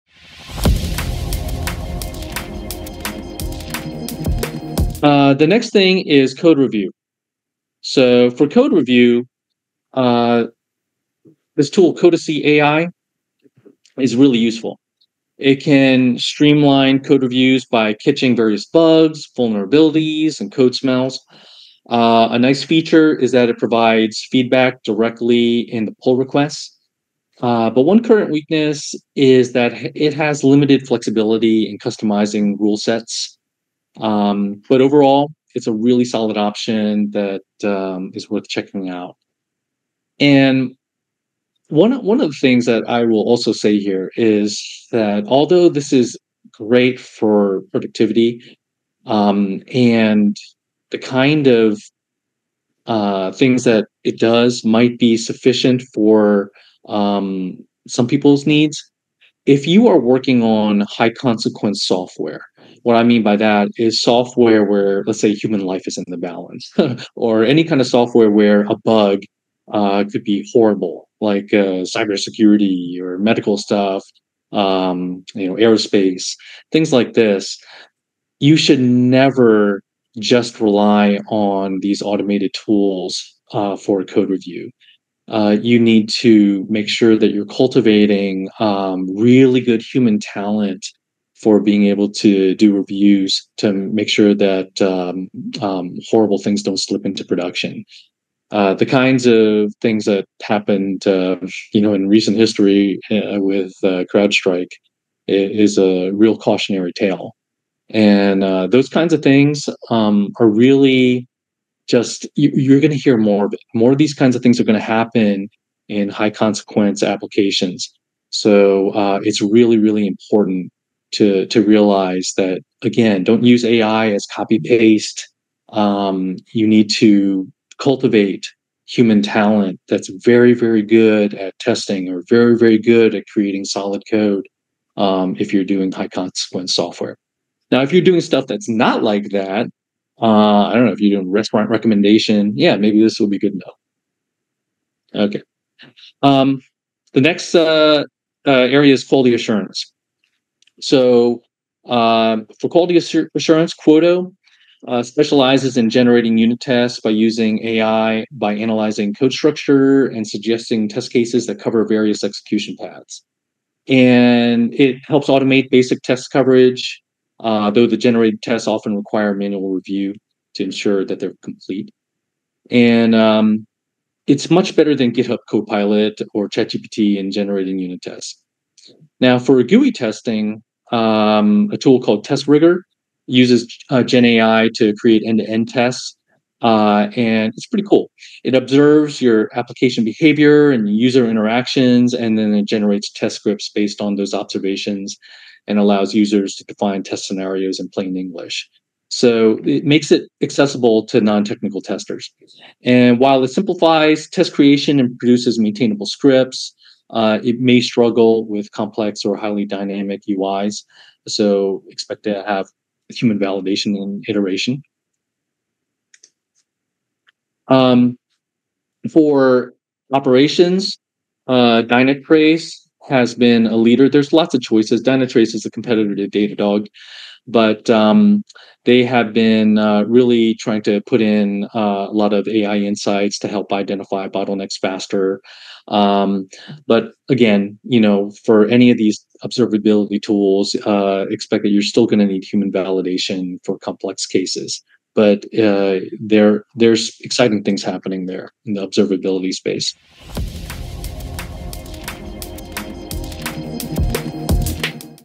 Generative AI for Software Developers: Code Review, Quality Assurance, Operations
Andrew Park explores AI-driven tools like CodeSee AI, Quota, TestRigor, and Dynatrace, highlighting their potential to streamline code reviews, automate testing, and enhance observability. He emphasizes the importance of human oversight in high-stakes software development while showcasing how these tools can boost productivity in less critical applications. A practical guide for developers navigating AI’s impact on software development.

In the mind map shown in this video, any blocks in red are what GenAI can highly displace (or, you could say, highly accelerate). What's in yellow are the things that GenAI can moderately displace (moderately accelerate). And what's in green are things that largely the human still has to do.